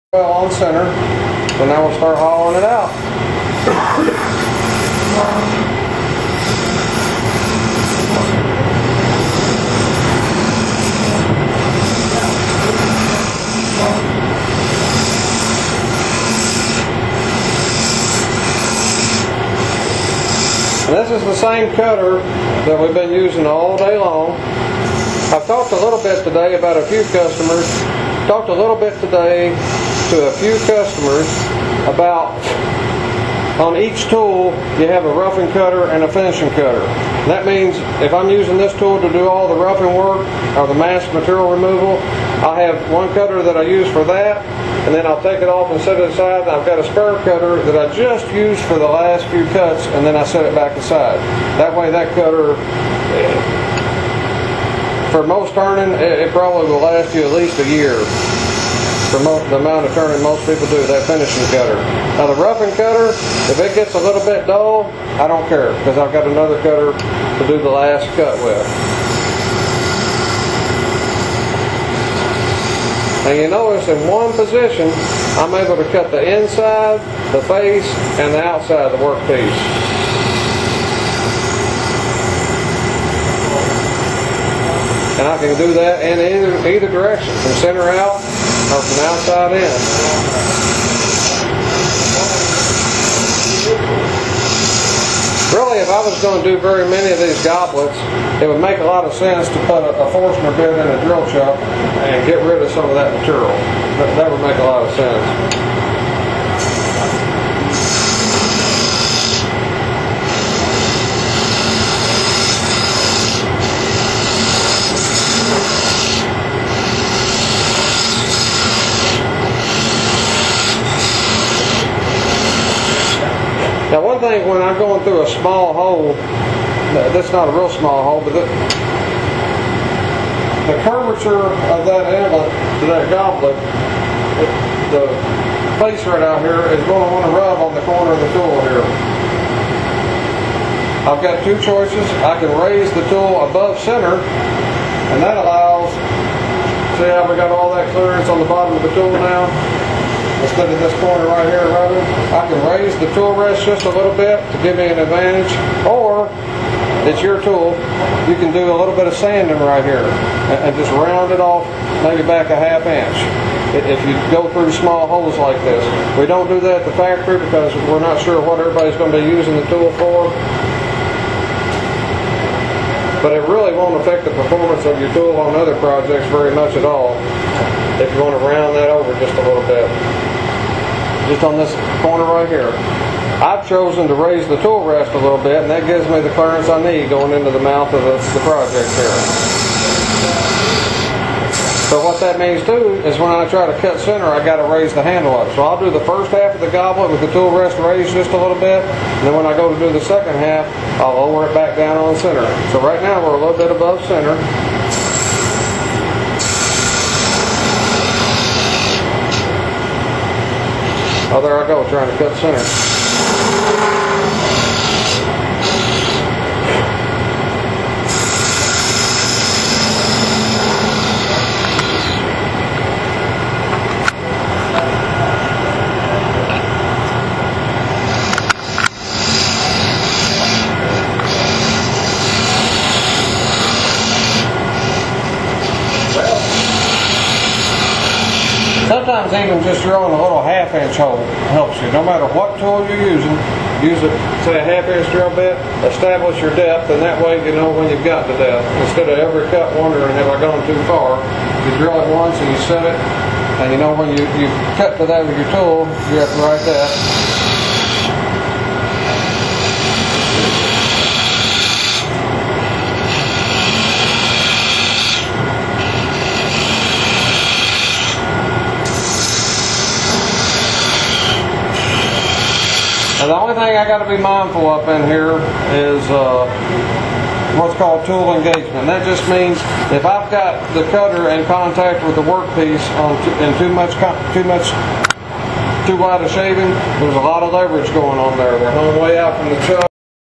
Easy Wood Tools -Goblet with Mini Tools Nov 09
Kiln Dried Maple - Craig Jackson doing a Goblet Demo using the Ci2m Mini Rougher and the Ci3m Mini Finisher available from Easy Wood Tools.  to purchase or for more information and a list of Authorized Retailers in your area.